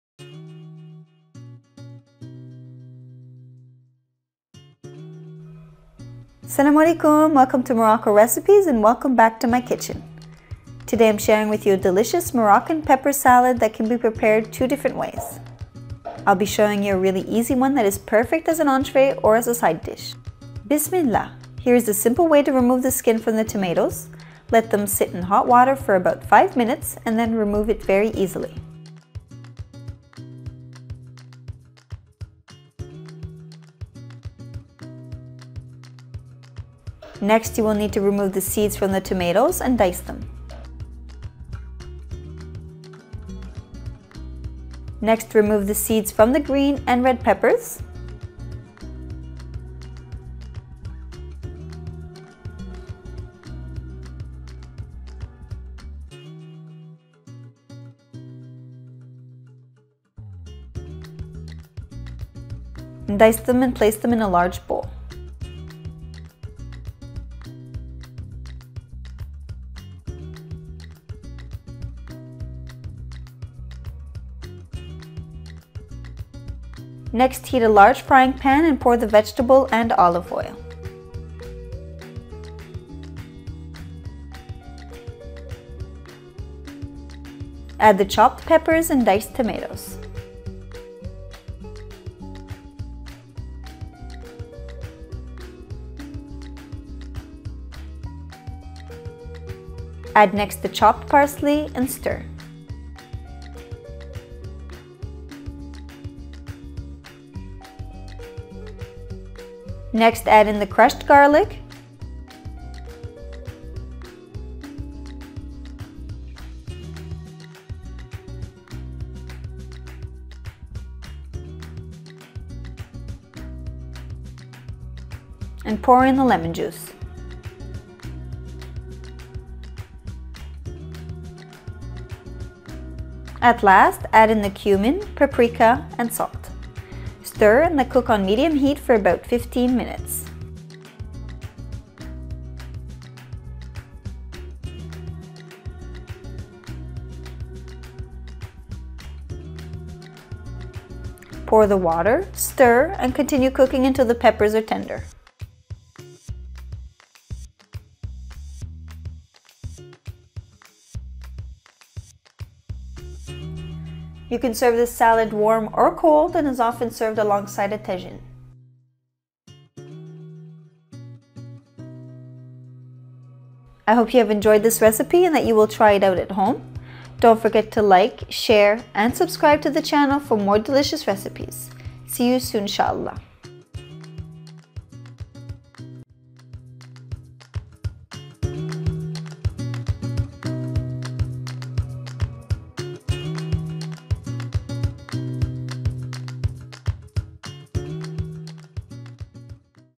Moroccan Pepper Salad (VEGAN) - Morocco Recipes
Easy, delicious moroccan peper salad that is perfect as an entree or as a side dish. Enjoy !

The more we reach people, the more we can continue doing videos! Thanks.

To print out all our recipes and for FREE, visit our

Moroccan Pepper Salad